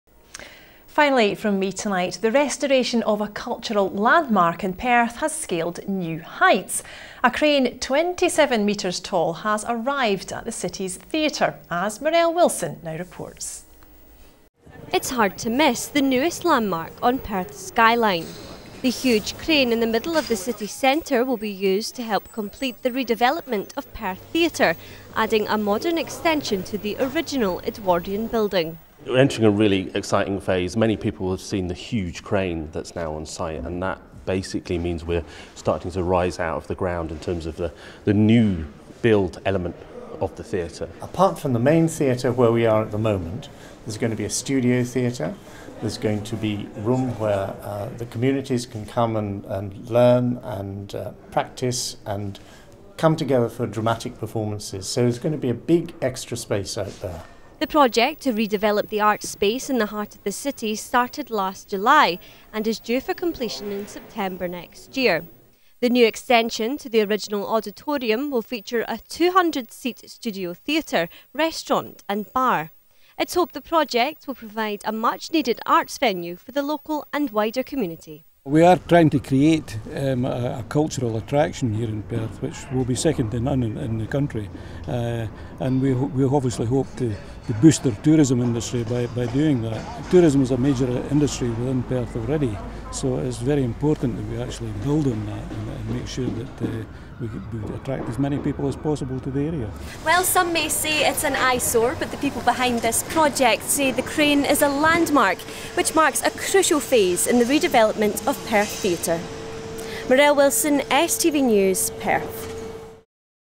Perth theatre development - STV News report
Thee restoration of a cultural landmark in Perth has scaled new heights... a crane - 27 metres tall - has arrived at the city's theatre, as Marelle Wilson reports.